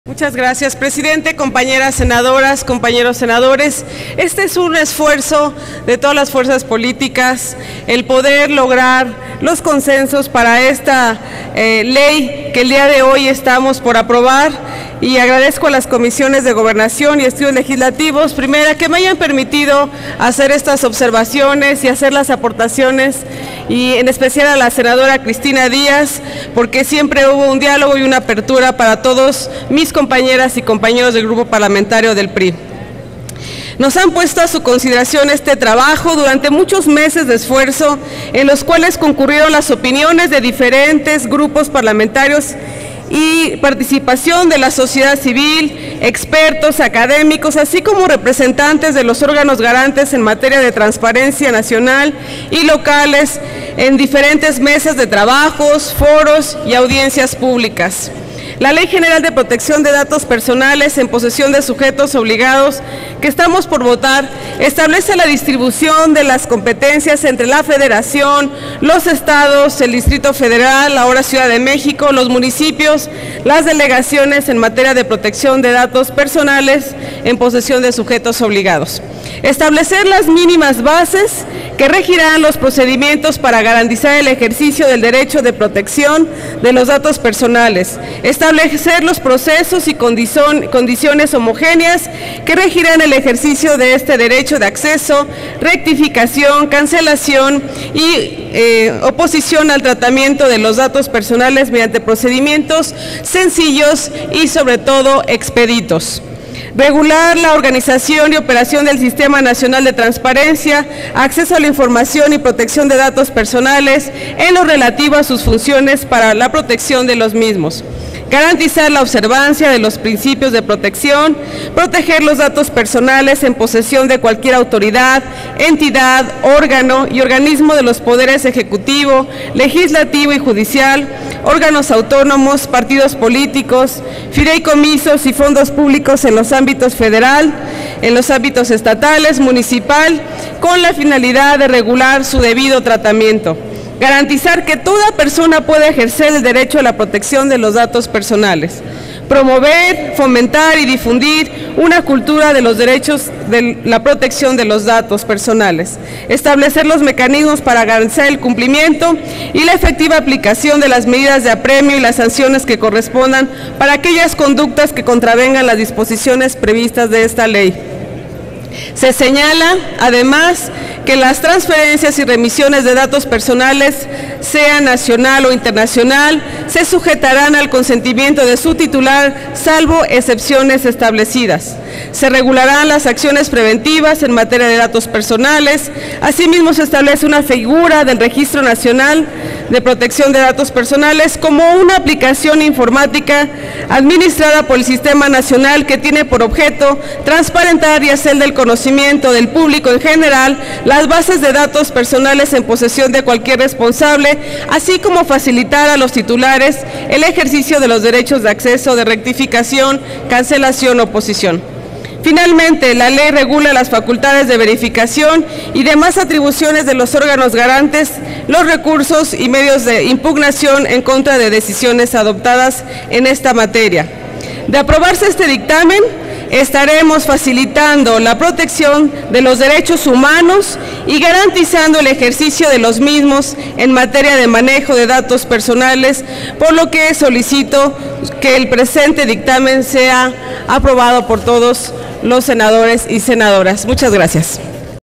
Promover y difundir una cultura de respeto a los datos personales: Senadora Lisbeth Hernández.
La Senadora del PRI, Lisbeth Hernández Lecona habló a favor del dictamen de la Ley General Protección de Datos Personales en Posesión de Sujetos Obligados.